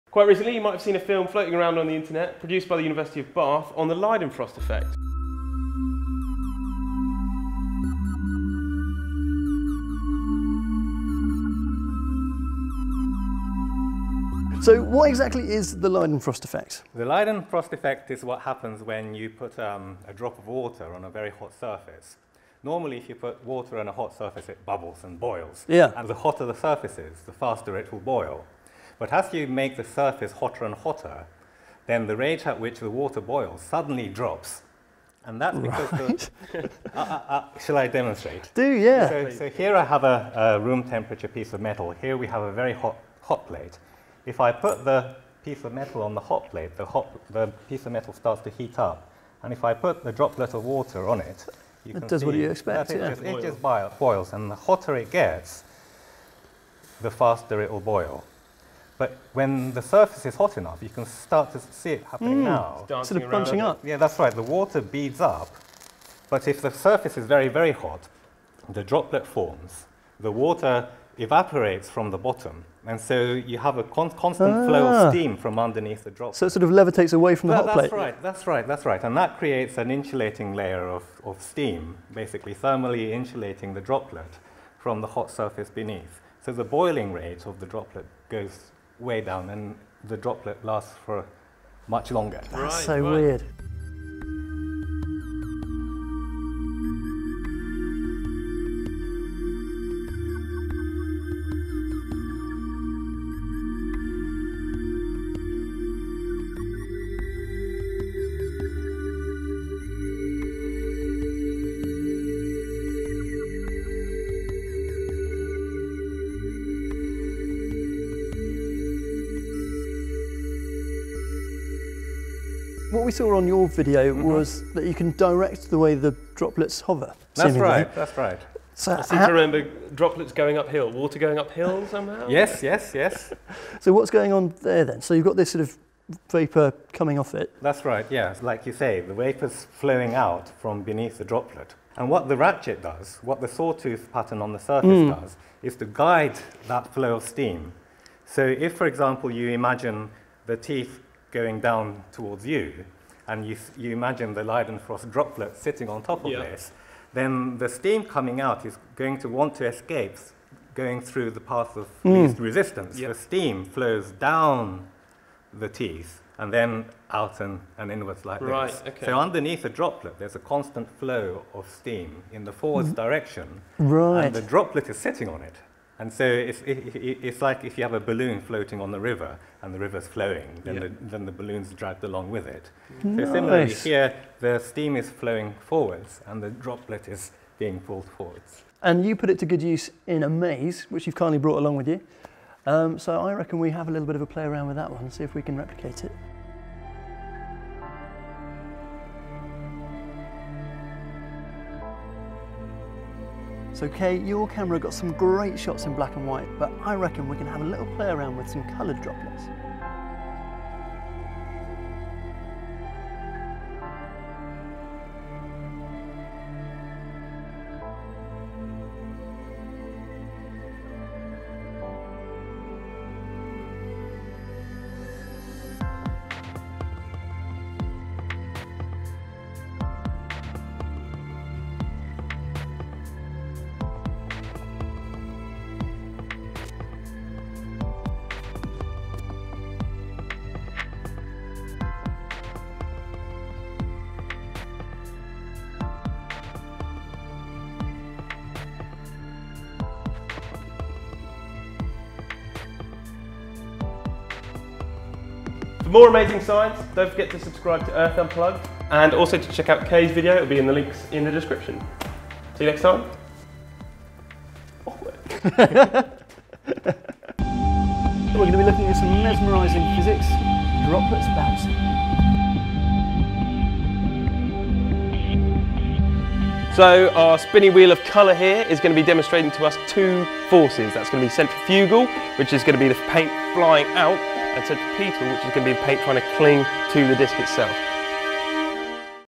Water Flows Uphill! Leidenfrost Effect | Slo Mo #41 | BBC Earth Explore
Following the recent success of the University of Bath's beautiful slow motion footage of the mysterious Leidenfrost effect, we invited Dr Kei Takashina to come and explain it all, and put our own twist on this incredible phenomenon.

Special thanks to Carmen Cheng, Matthew Guy and all the team from the University of Bath for allowing us to film the fruits of their labour.